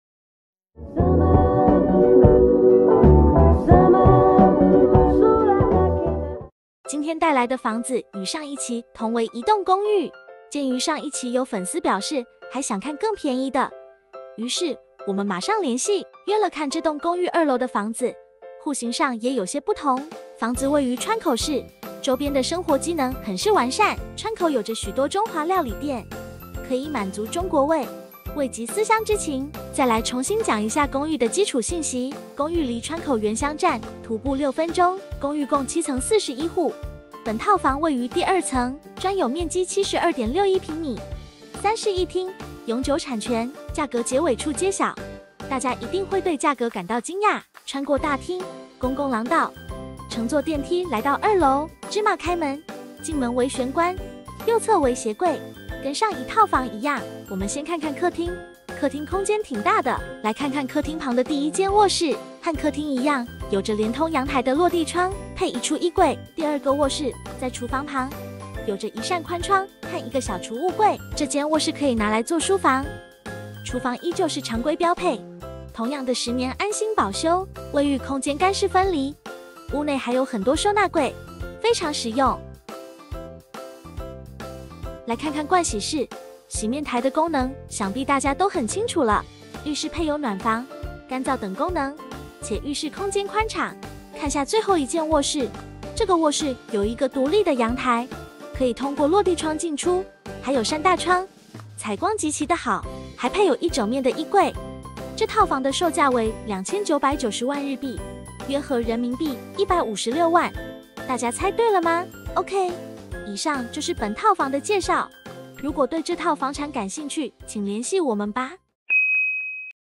RealEstate便宜到不敢信？| 川口市雙陽台公寓｜東京都市圈內｜中華料理HomeTour【日本房產】life
今天介紹的這套埼玉縣川口市的雙陽台房，與上一期視頻介紹的房子同位於該公寓中，但這套房位於2樓，價格更為便宜，周邊不僅有著許多活動場所，還可以品嚐到多樣的中華料理美食，公寓位於東京都市圈內，適合學生、上班族通勤！ 大家對套房有什麼問題，或是想看什麼樣的房產都歡迎在下面評論、留言哦！
➤ 聯系我們
微信：oceantokyo2（備註Youtube）
Line：oceantokyo818（備註Youtube）